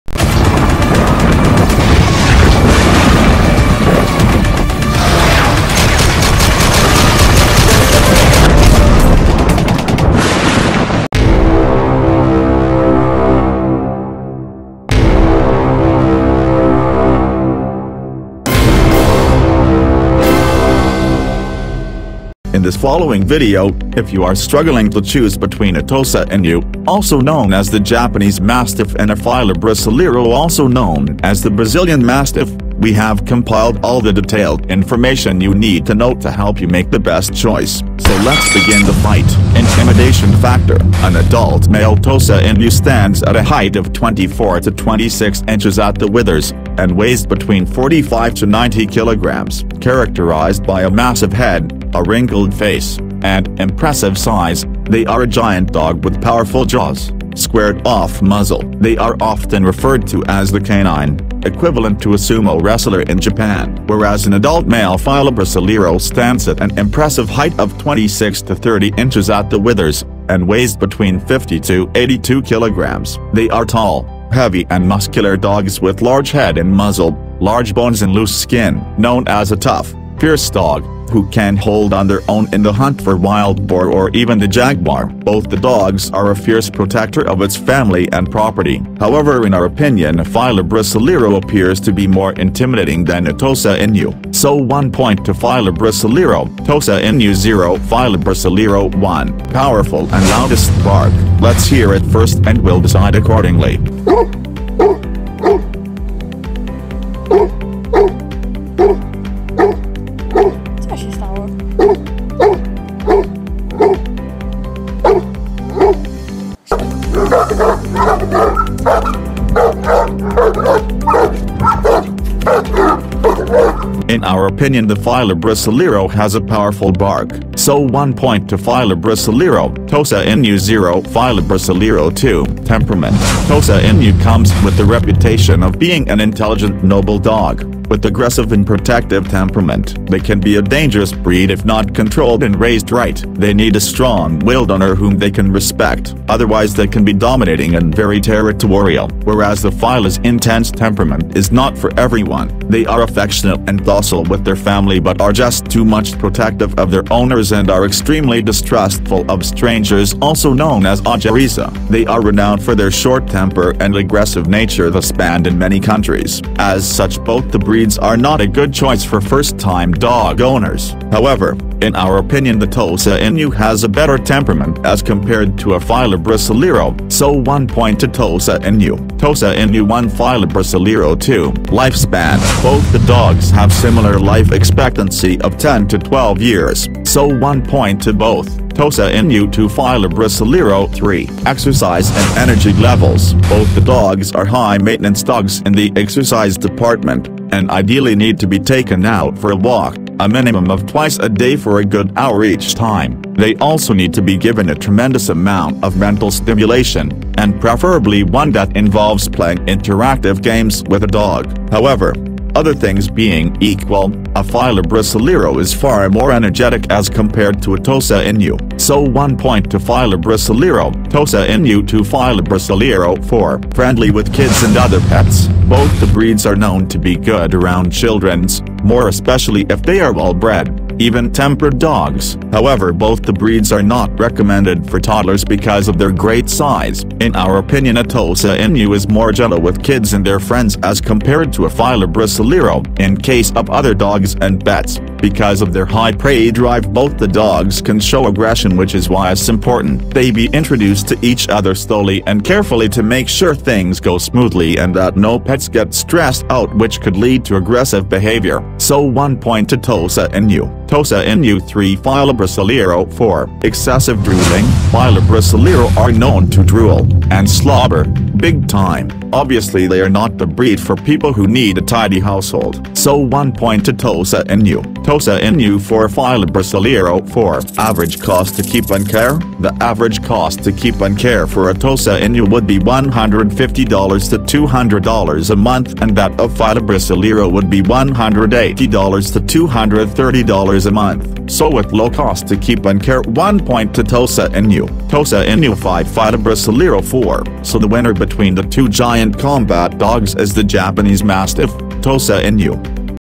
Tosa Inu vs Fila Brasileiro | Battle Between Combat dogs | Billa Boyka
Tosa Inu vs Fila Brasileiro | Japanese Mastiff vs Brazilian Mastiff | Battle between Blacklisted dogs | Billa Boyka |
In the following video, if you are struggling to choose between a Tosa Inu and a Fila Brasileiro. We have compiled all the detailed information you need to know to help you make the best choice. So lets compare these two large dogs who often appears in the top list of dangerous dogs worldwide based on below mentioned parameters
Tosa Inu vs Fila Brasileiro intimidation factor
Tosa Inu vs Fila Brasileiro powerful bark,
Tosa Inu vs Fila Brasileiro temperament,
Tosa Inu vs Fila Brasileiro exercise and energy levels
Tosa Inu vs Fila Brasileiro life span,
Tosa Inu vs Fila Brasileiro friendly with kids and other pets,
Tosa Inu vs Fila Brasileiro cost to keep,
Tosa Inu vs Fila Brasileiro for first time owners,

Video Credit:
From 1:04 to 1.10 The dog that appears standing with a woman at the beginning of the video is Maximus from ITINGA, from the channel "kennel ITINGA Fila brasileiro".
Canil Itinga Fila Brasileiro Kennel

We didnt have the bite strength data for Fila Brasileiro hence not considered for making the comparison.